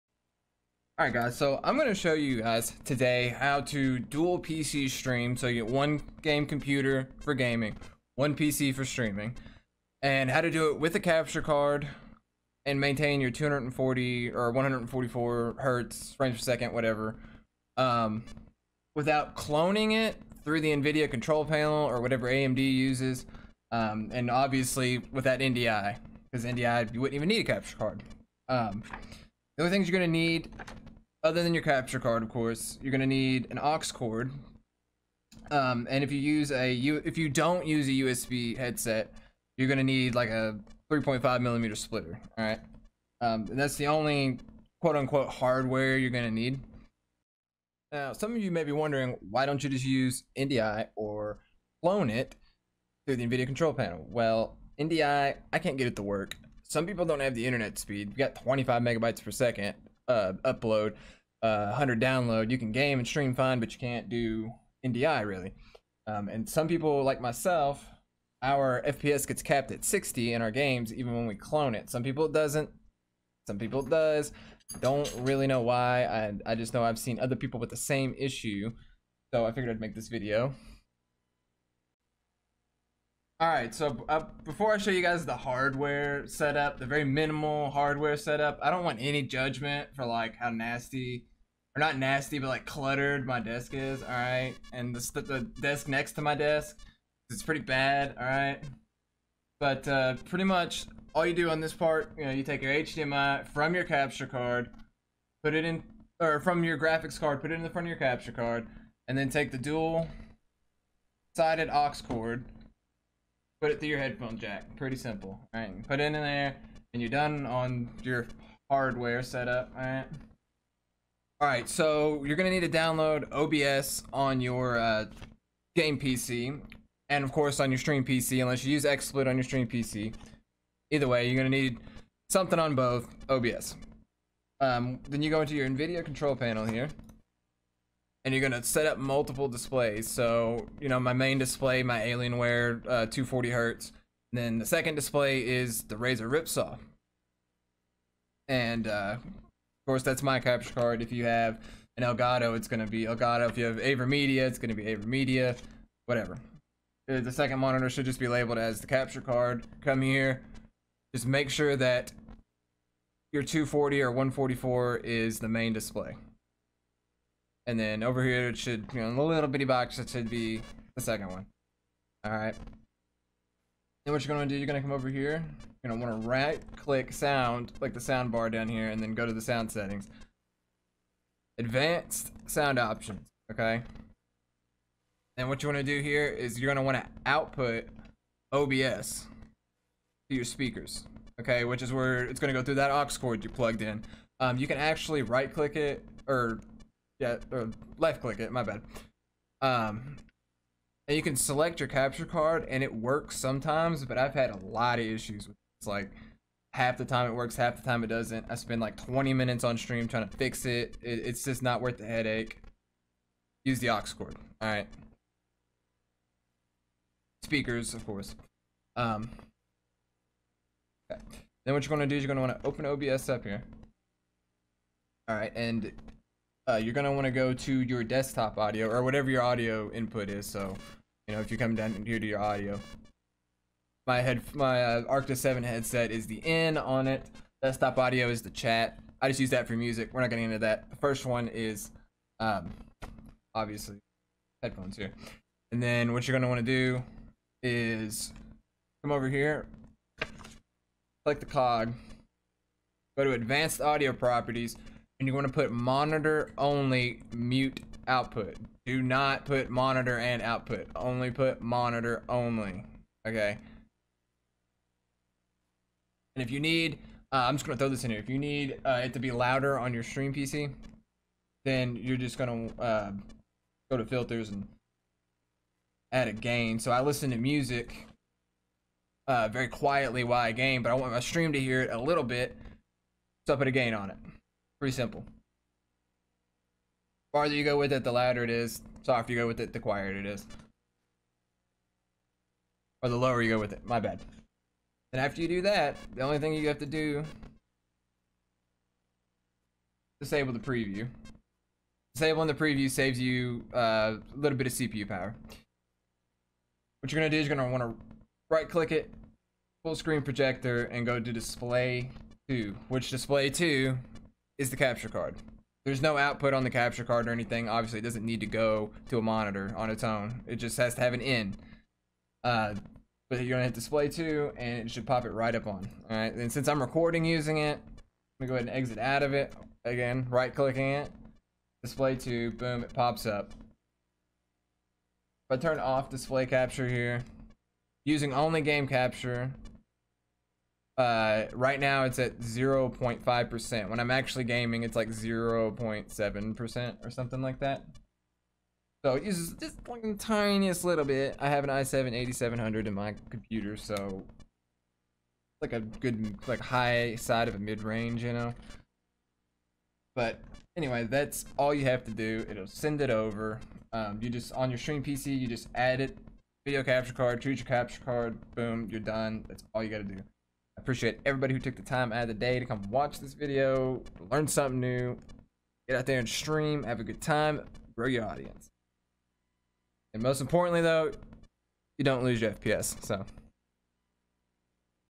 How to Dual PC Stream NO NDI OR CLONING - Maintain 144fps+ on one PC while Streaming on the other
In this video I cover how to use One PC for Gaming and a Second PC to stream without using NDI or without cloning through the Nvidia control panel so you can maintain your high frame rates. Someone people can clone fine, others can't and if you can't then here is the solution! Even though you don't clone, you will still need a capture card!
Well I stream on that place Ninja switched to!